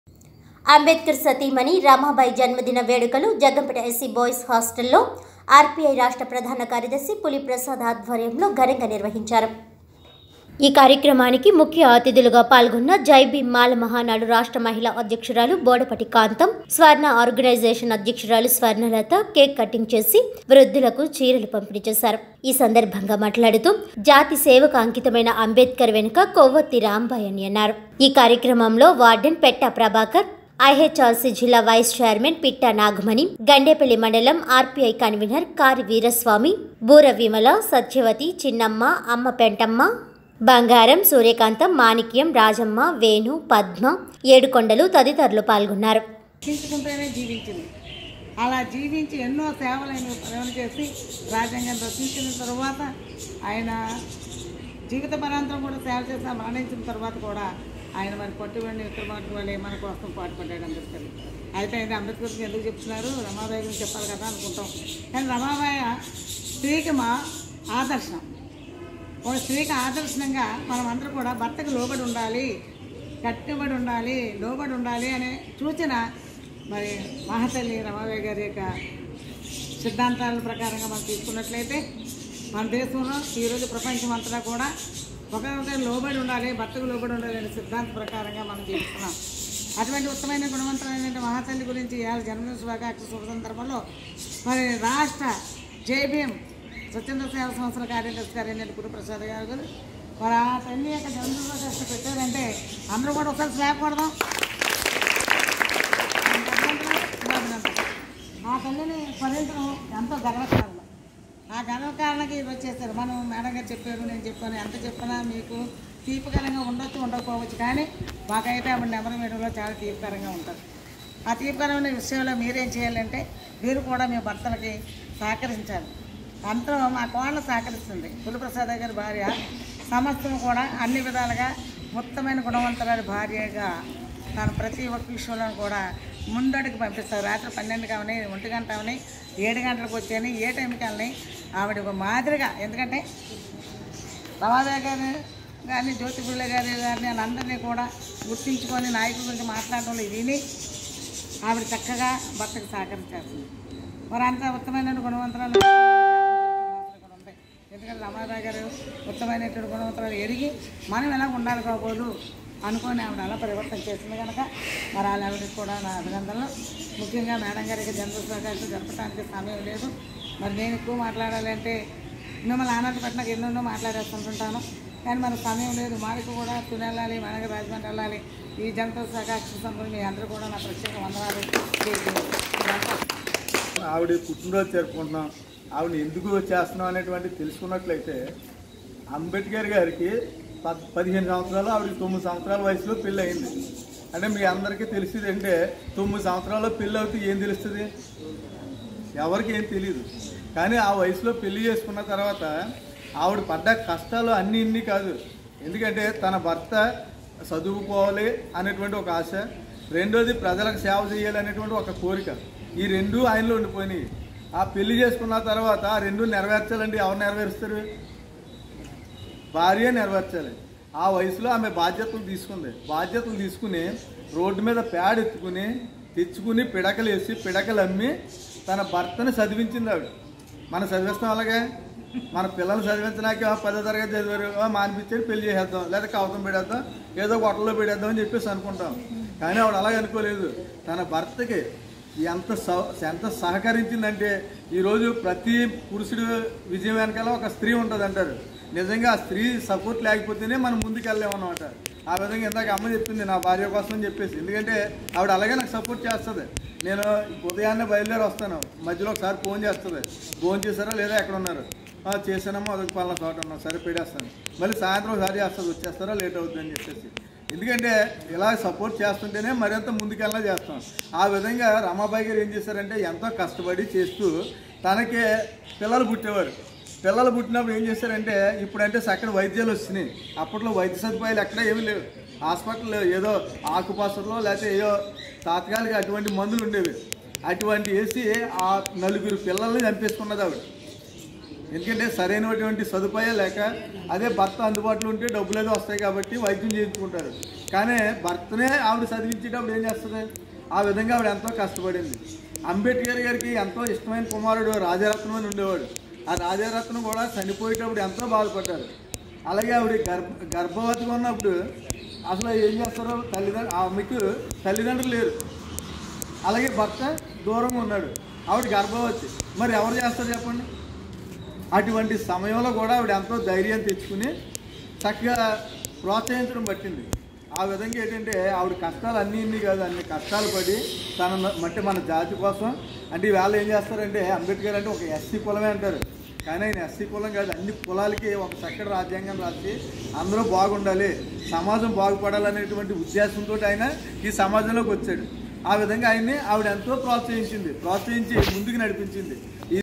జగ్గంపేట- ఘనంగా రమాబాయ్ జయంతి _|TV7 NEWS|08-02-2021|EGDT|AP
అంబేడ్కర్ సతీమణి రమాబాయ్  జన్మదిన వేడుకలు జగ్గంపేట ఎస్సై బాయ్స్ హాస్టల్లో  ఆర్ పి ఐ రాష్ట్ర ప్రధాన కార్యదర్శి పులి ప్రసాద్ ఆధ్వర్యంలో ఘనంగా నిర్వహించారు.